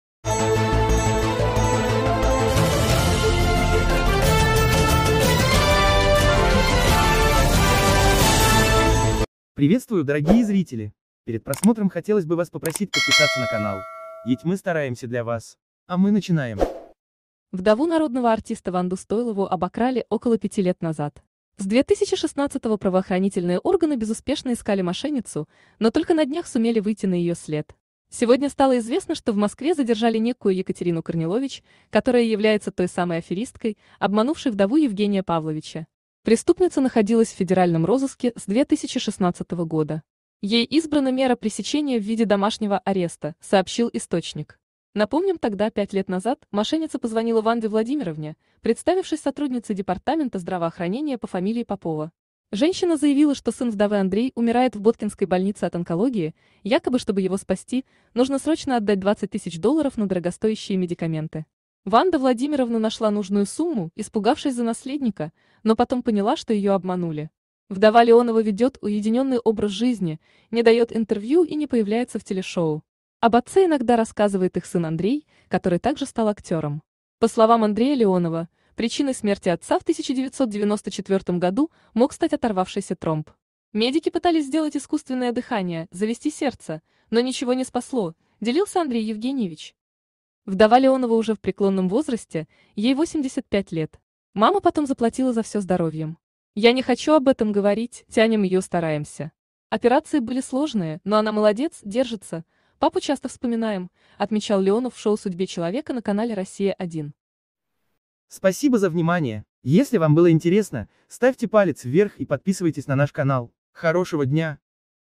Задержана женщина, обокравшая вдову Евгения Леонова
Вдову народного артиста Ванду Стойлову обокрали около пяти лет назад. С 2016-го года правоохранительные органы безуспешно искали мошенницу, но только на днях сумели выйти на ее след.